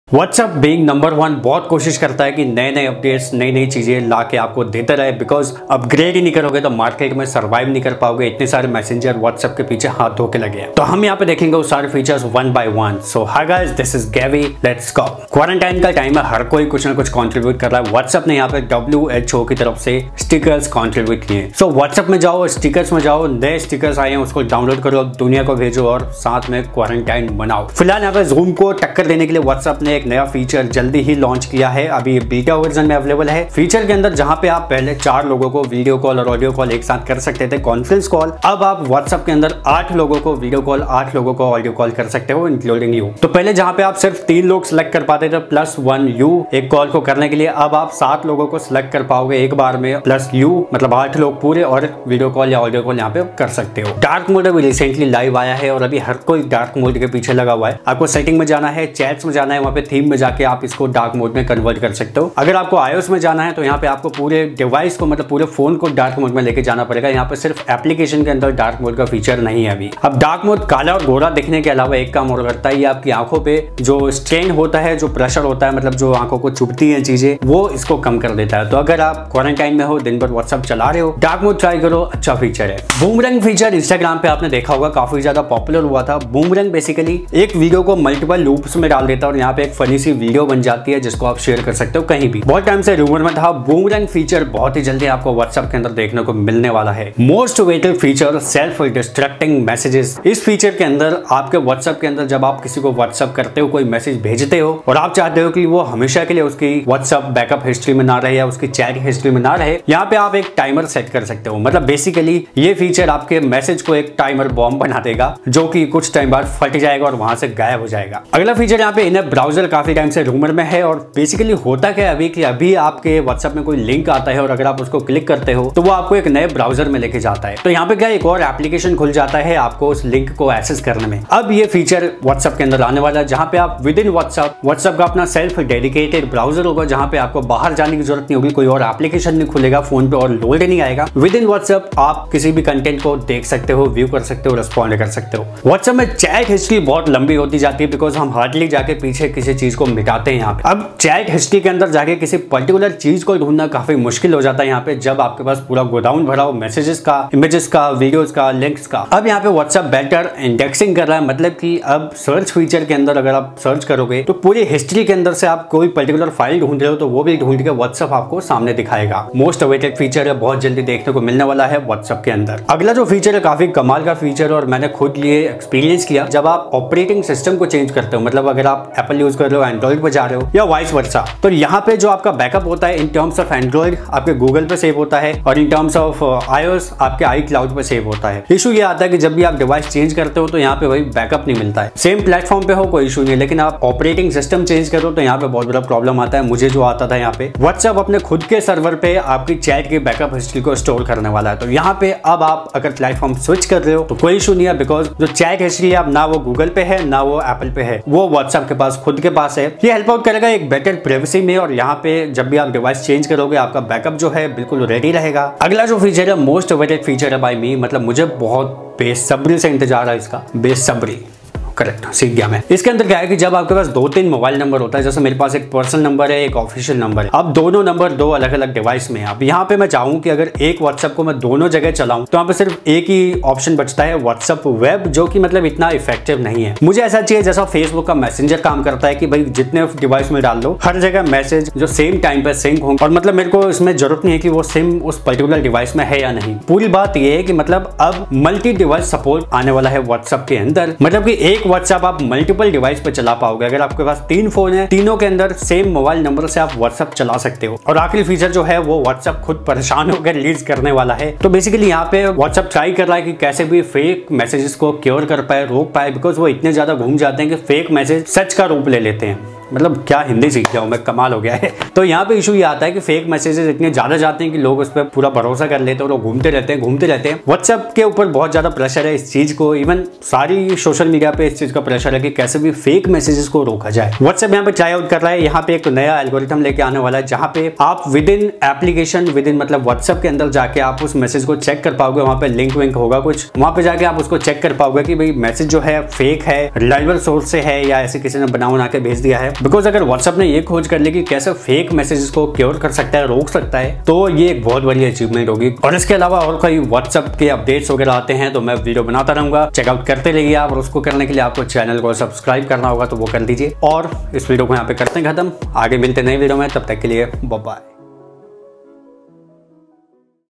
WhatsApp New Features | Whatsapp Upcoming🔥 बारूद 🔥Features ! May 2020
🙏 Namashkar Fellows 🙏

Iss video mai aapse baat karne wala hoon Whatsapp New and Whatsapp Upcoming Features ke baare main !!

Explained :-
1. Whatsapp Latest features 2020
2. Whatsapp upcoming features
3. Whatsapp future updates and features
4. Whatsapp 8 people group call feature
5. Whatsapp Boomrang update
6. Whatsapp Stickers
7. Whatspp self destructing message feature
8. Whatsapp in app browser update
9. Whatsapp dark more in android
10. Whatsapp dark mode in ios
11. Whatsapp multiple device support i.e run one whatsapp on many phones without whatsapp web
12. Whatsapp cool features

Camera : Canon 80D
Mic : Snowball White/Clip Mic
Editor : Final Cut Pro X
Laptop : MacBook Pro
Sound Editor : Audacity

Namaskar Fellows 🙏